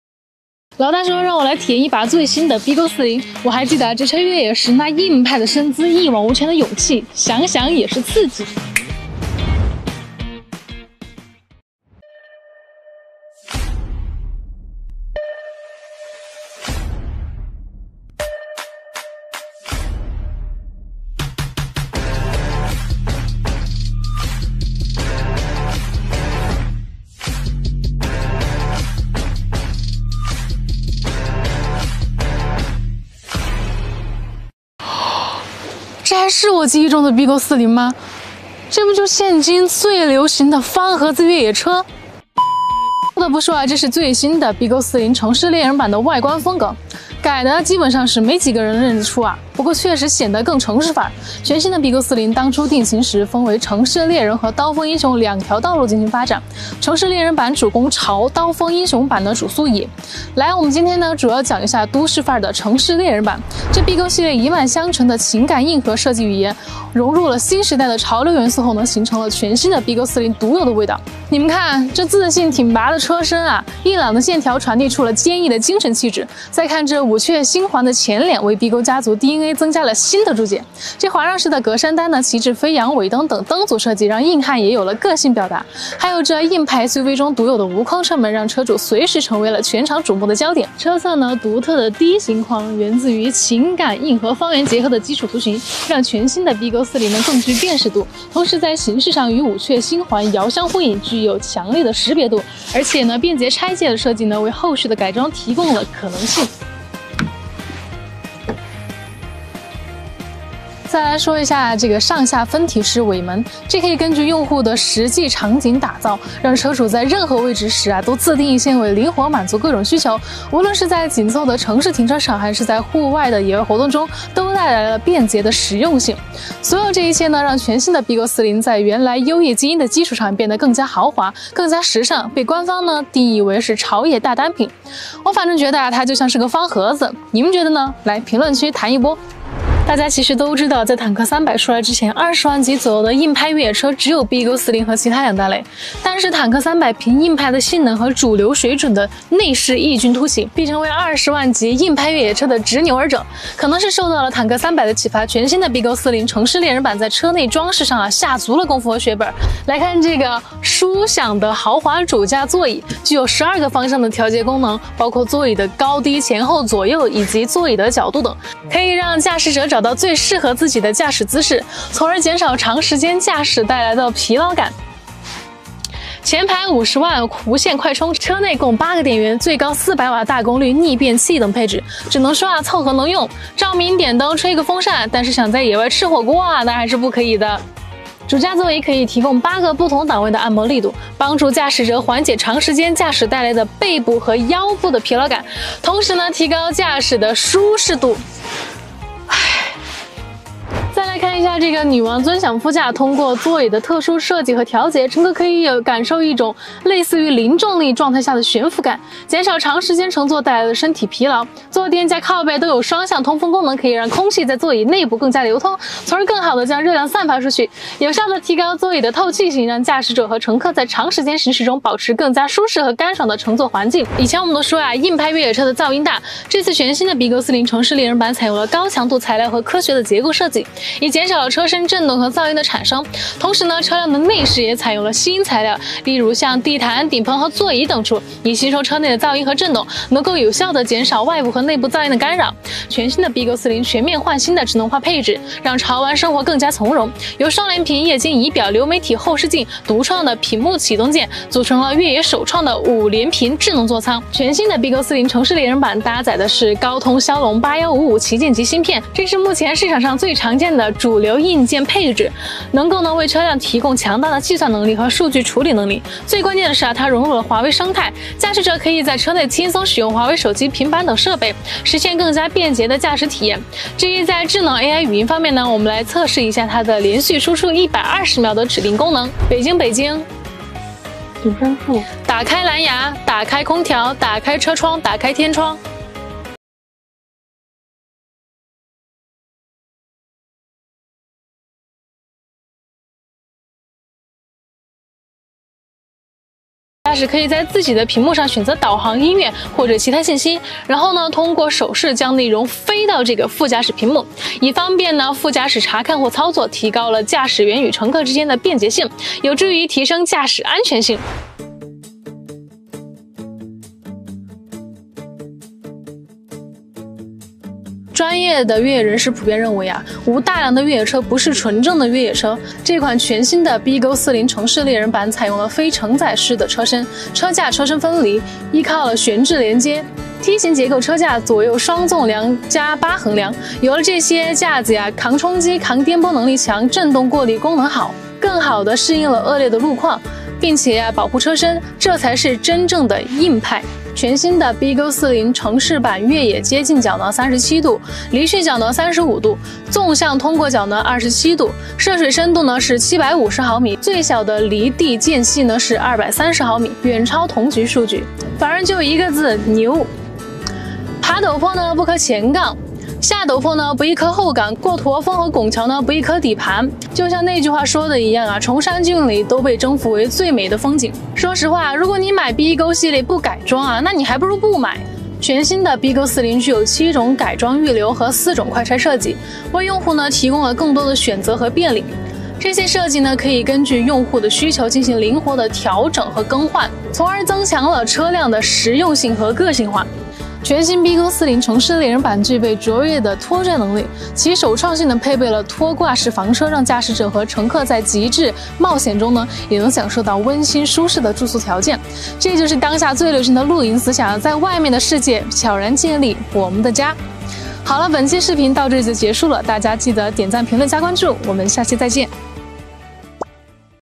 美女试驾全新BJ40城市猎人版：非承载就是硬
我们的美女主播带大家来试驾全新BJ40城市猎人版啦！给你点出几个心得小tips
车系： 北京BJ40